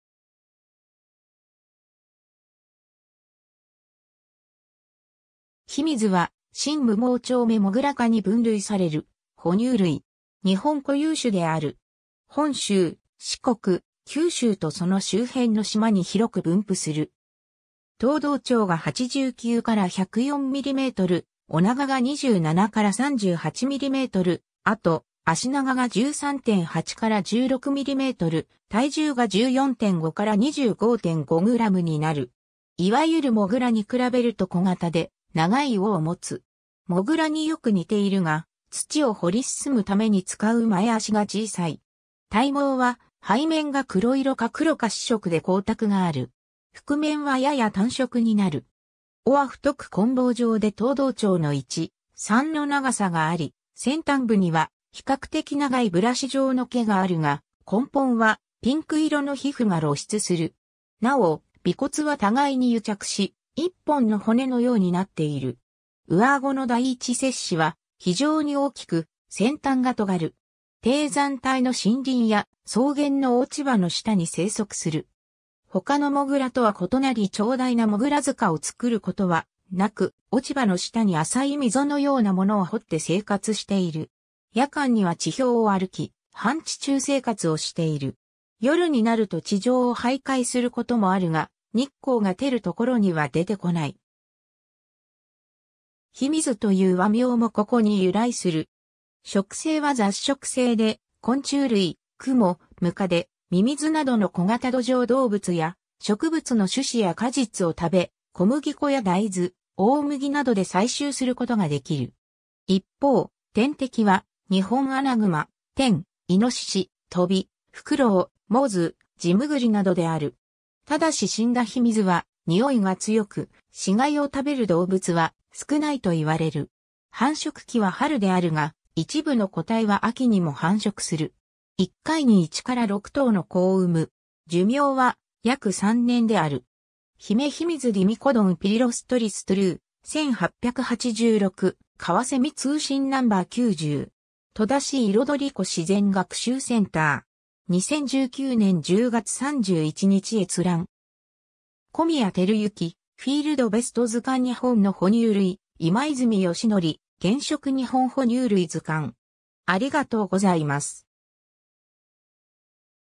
ヒミズ
ヒミズ（日不見、日見ず 学名：Urotrichus talpoides）は、真無盲腸目モグラ科に分類される哺乳類。
 日本固有種である。
本州、四国、九州とその周辺の島に広く分布する。
 頭胴長が89-104mm、尾長が27-38mm、後足長が13.8-16mm、体重が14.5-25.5gになる。
いわゆるモグラに比べると小型で、長い尾をもつ。
モグラによく似ているが、土を掘り進むために使う前足が小さい。
体毛は背面が黒色か黒褐色で光沢がある。
腹面はやや淡色になる。
尾は太く棍棒状で頭胴長の1/3の長さがあり、先端部には比較的長いブラシ状の毛があるが、根本はピンク色の皮膚が露出する。
なお、尾骨はたがいに癒着し、1本の骨のようになっている。
上顎の第1切歯は非常に大きく、先端が尖る。
 低山帯の森林や草原の落ち葉の下に生息する。
他のモグラとは異なり長大なモグラ塚を作ることはなく、落ち葉の下に浅い溝のようなものを掘って生活している。
 夜間には地表を歩き、半地中生活をしている。
夜になると地上を徘徊することもあるが、日光が照る所には出てこない。
「日見ず」という和名もここに由来する。
 食性は雑食性で、昆虫類、クモ、ムカデ、ミミズなどの小型土壌動物や植物の種子や果実を食べ、小麦粉や大豆、大麦などで採集することができる。
 いっぽう、天敵はニホンアナグマ、テン、イノシシ、トビ、フクロウ、モズ、ジムグリなどである。
ただし死んだヒミズは臭いが強く、死骸を食べる動物は少ないといわれる。
 繁殖期は春であるが、一部の個体は秋にも繁殖する。
1回に1-6頭の仔を産む。
寿命は約3年である。
 ヒメヒミズ Dymecodon pilirostris True, 1886 ^ “カワセミ通信 No.90”. 戸田市彩湖自然学習センター. 2019年10月31日閲覧。
 ^ 小宮輝之 『フィールドベスト図鑑 日本の哺乳類』 ^ 今泉吉典 『原色日本哺乳類図鑑』